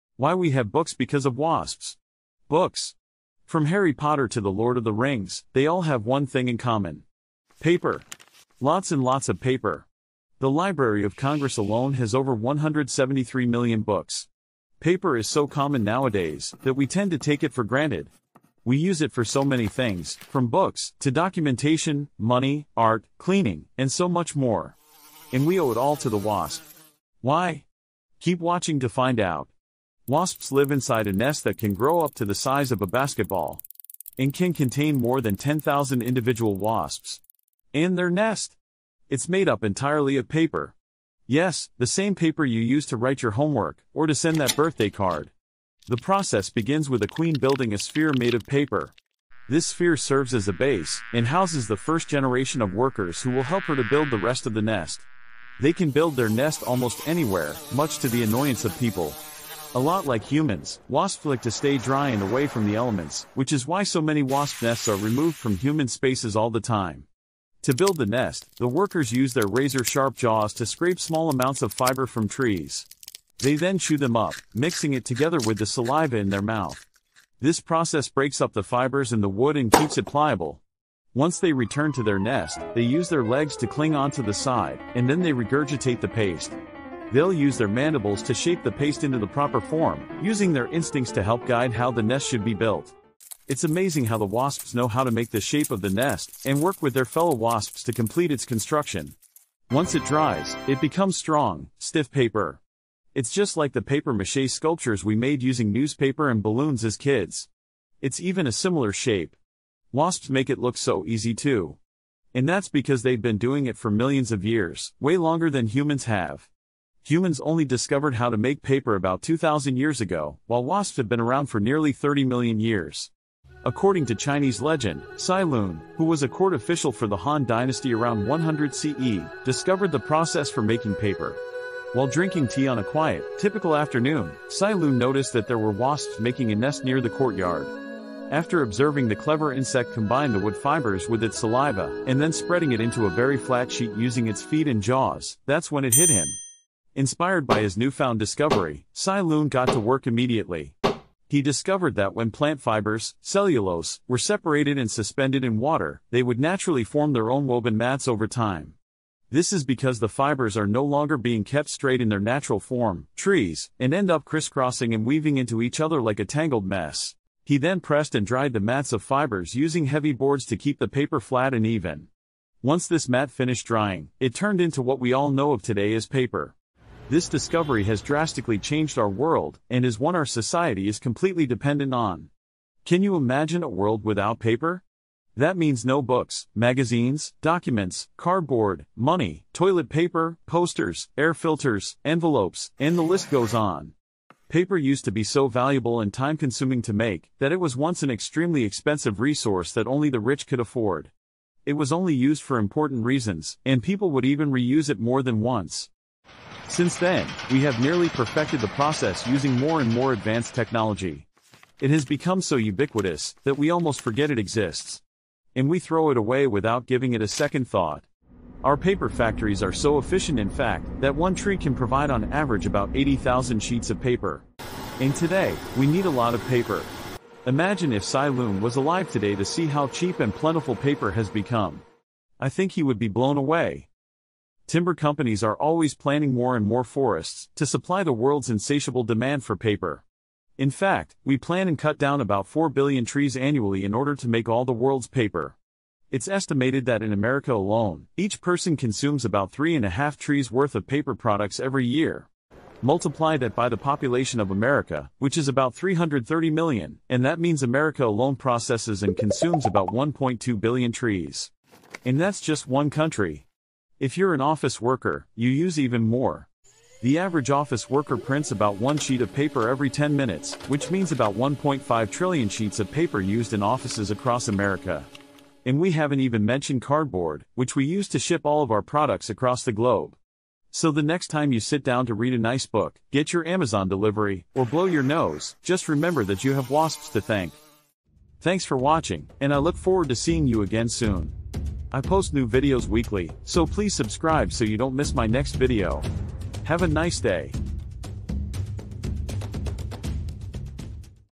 Why we have BOOKS because of WASPS!
Watch the amazing story of why we have books because of wasps! Humans have been making paper for a few thousand years, but that's nothing compared to how long wasps have been doing it! Watch to learn how we discovered the process of making paper, and why our society is so dependent on the common, everyday item. Our world has been transformed because of paper, and it's all thanks to the inspiration from Wasps!